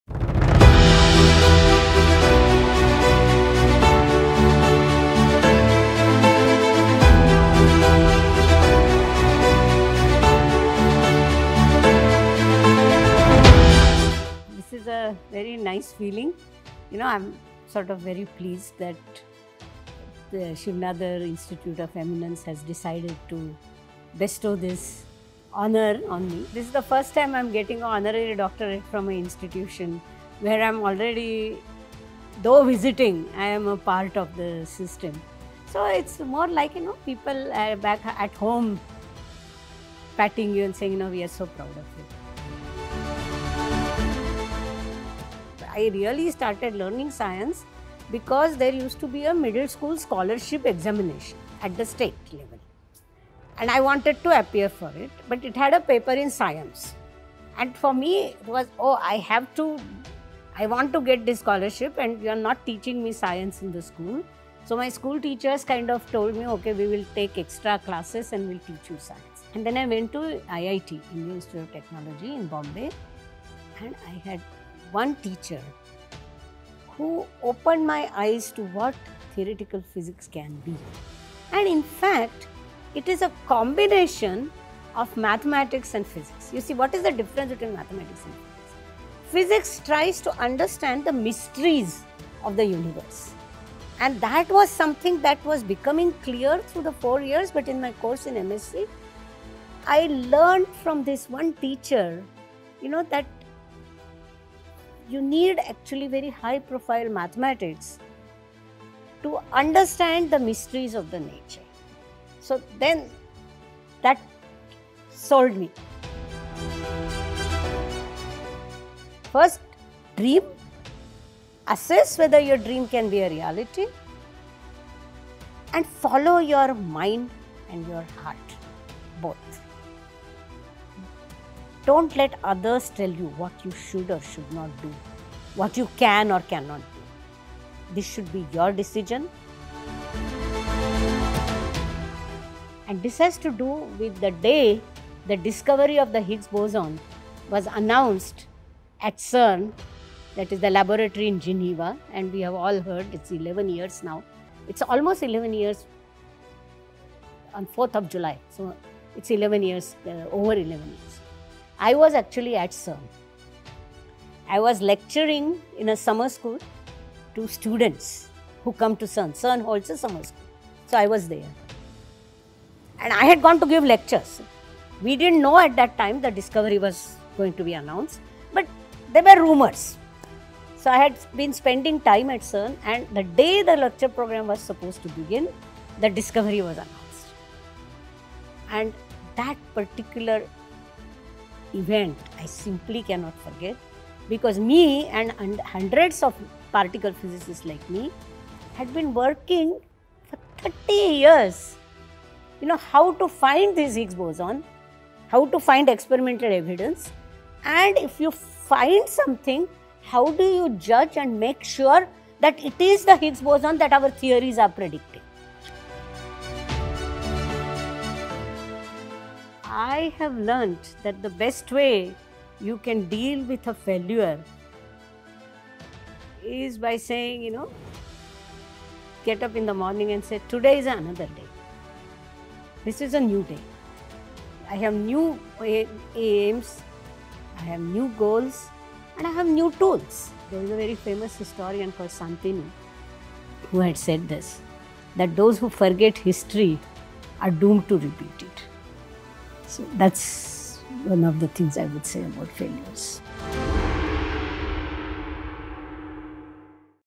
Convocation 2023 | In conversation with Prof. Rohini Godbole
In conversation with Prof. Rohini Godbole, Padma Shree Awardee and noted Indian Physicist. She talks about her decision to pursue theoretical physics, the ‘eureka’ moment in her career, and her secret to coping with failure. "It is better to let go of your failures but not forget them. You need to learn from them. In research, this happens quite often and the trick lies in understanding the processes of science to realize when you should let go of an idea. That’s something you learn as you go along," she shares.

Prof. Godbole was the Guest of Honor and Honorary Doctorate recipient at the 9th Convocation Ceremony of Shiv Nadar Institution of Eminence.